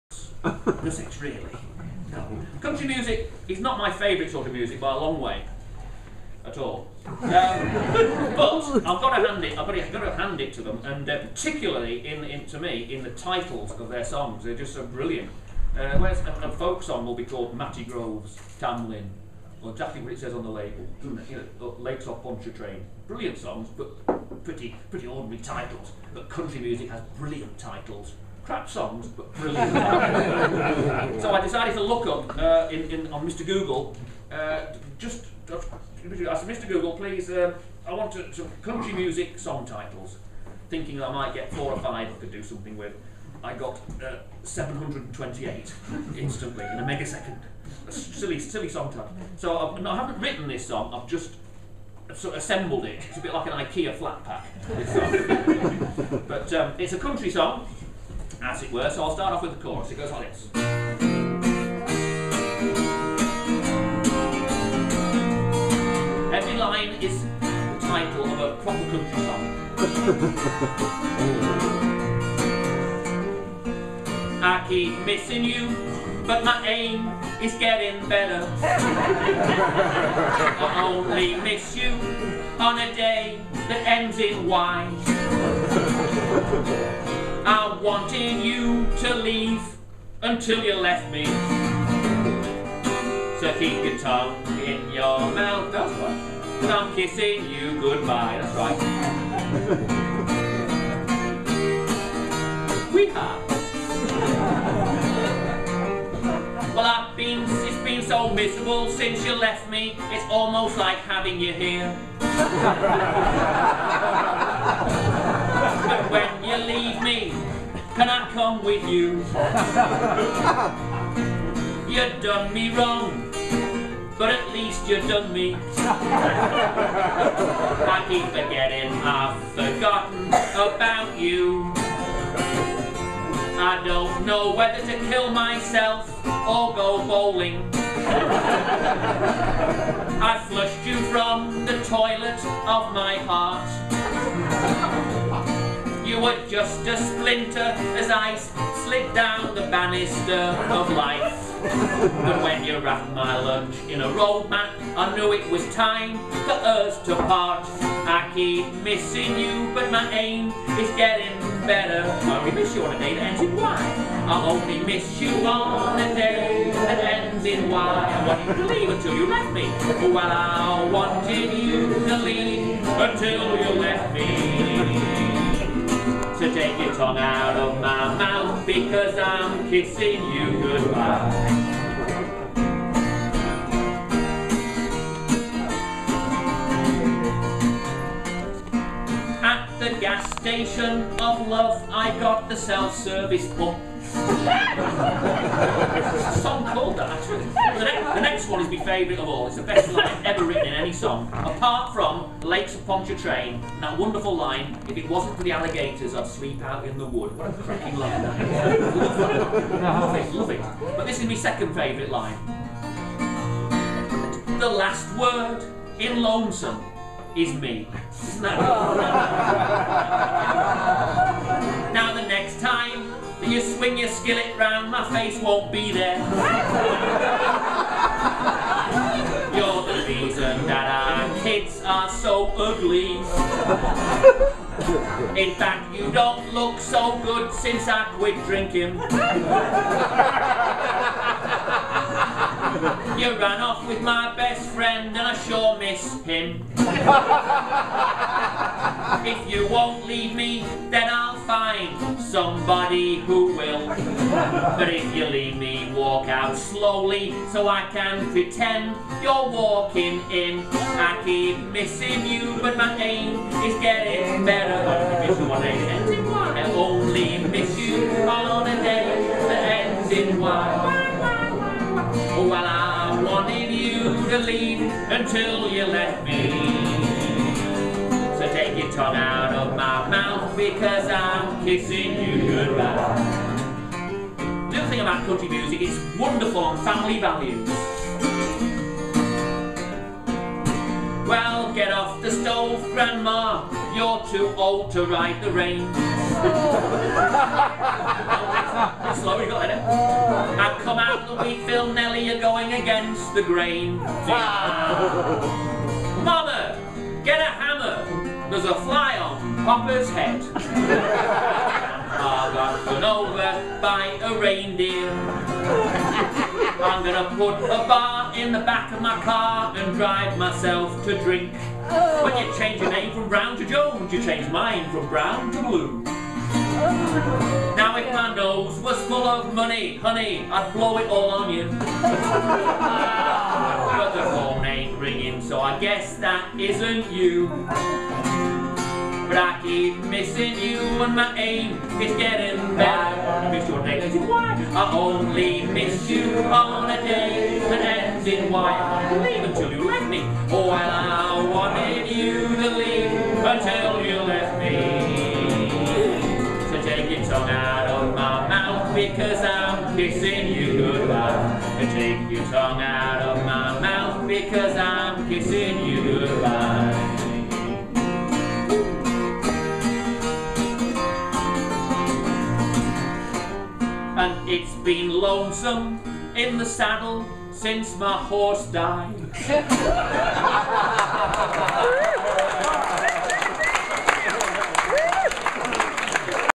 Stanley Accrington on Country Music - Chorlton Folk Club
Chorlton Folk Club -
Part of Chorlton Arts Festival
Live without mics or amps
upstairs at SW Manchester Cricket Club
Every Thursday from 9:00 pm
May 2010